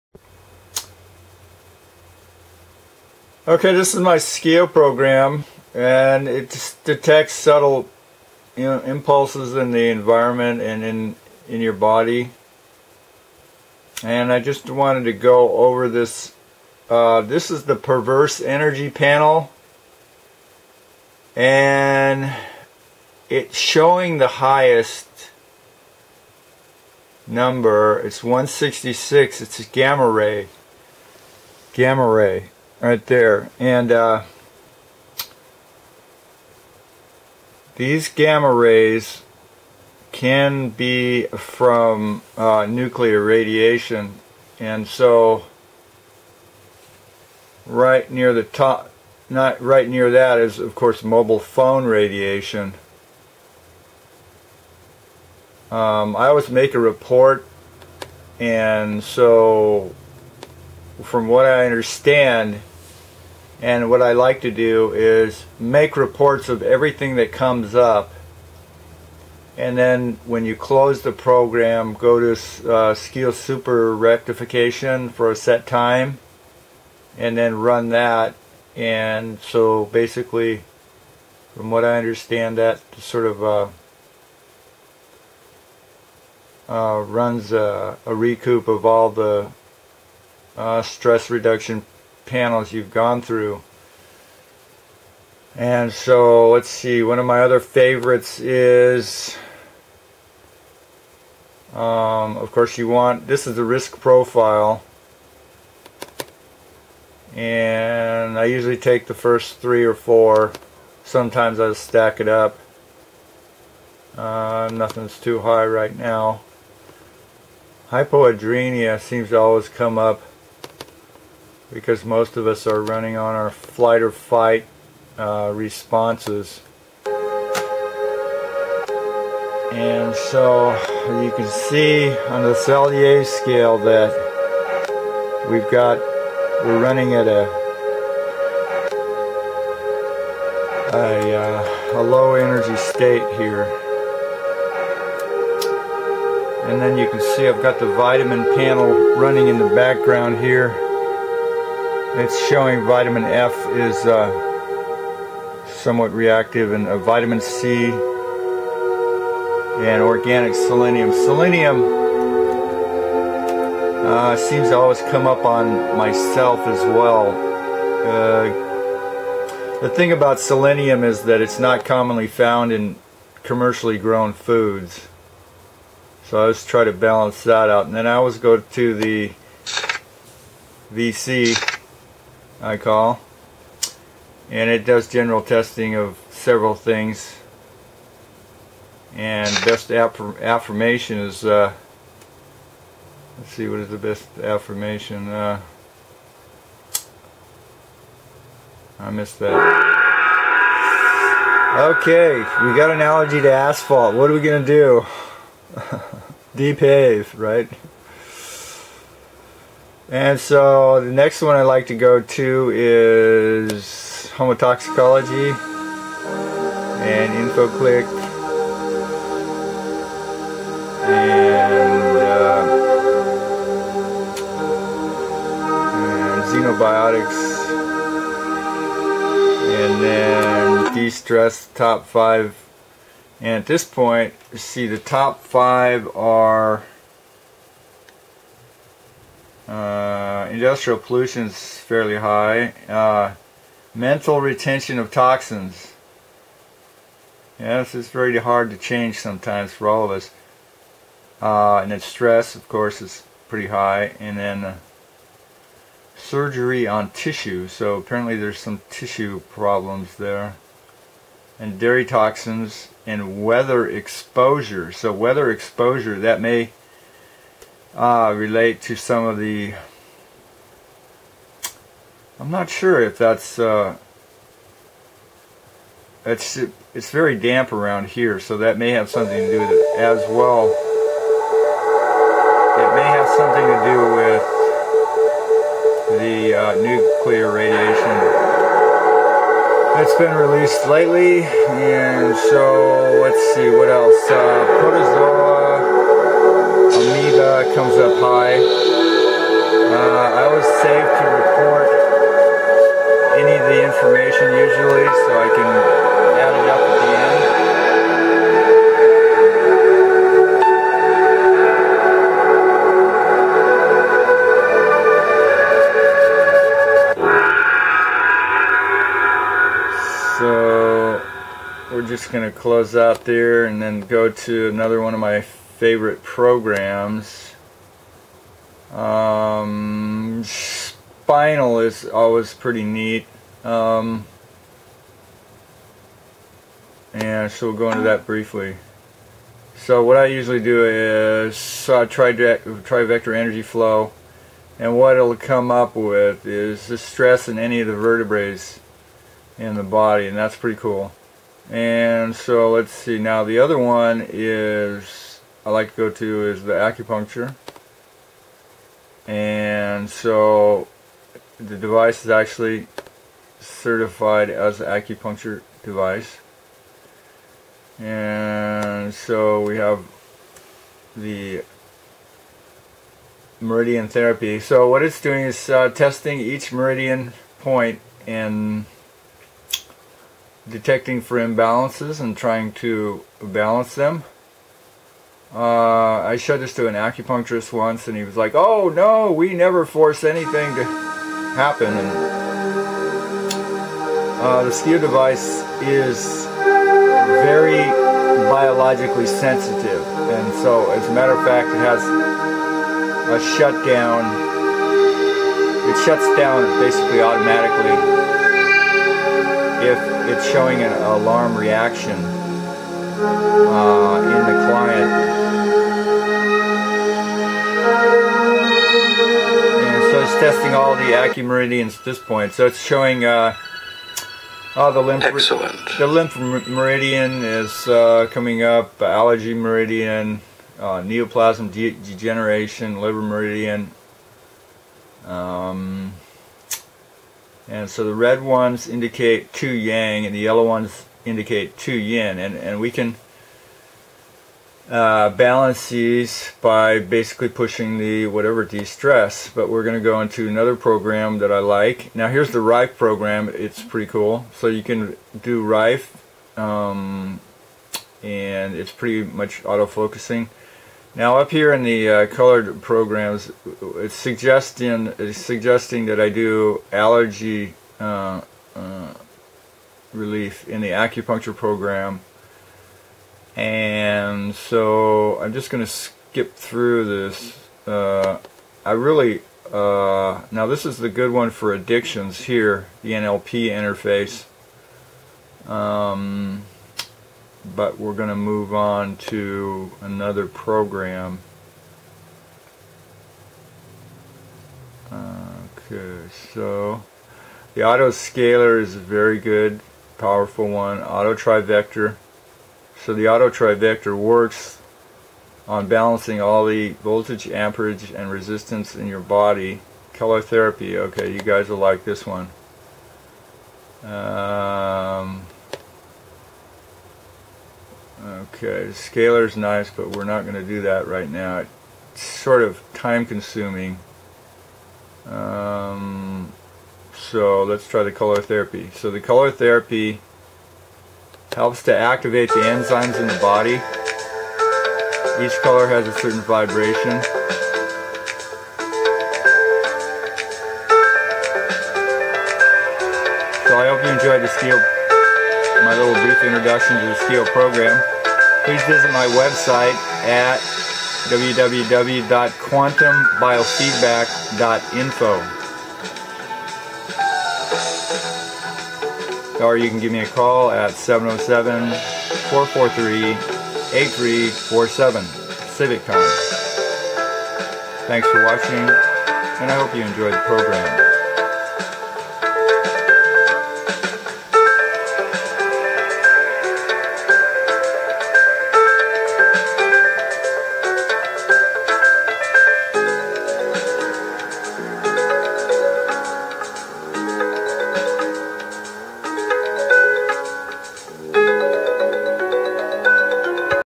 Quantum Biofeedback Research Community of Ca 707 443-8347
Working with the largest stress reduction computerized program in the world. I was trained as traditional medical assistant and became rebellious to what I experienced as a lack of preventative health care intervention. Than I found the SCIO . It is an amazing device! I have been training for 4 years now and still am not familiar with all the functions.Here I share with you some of my favorite panels. The system is entirely mobile and I can carry everything in my back pack.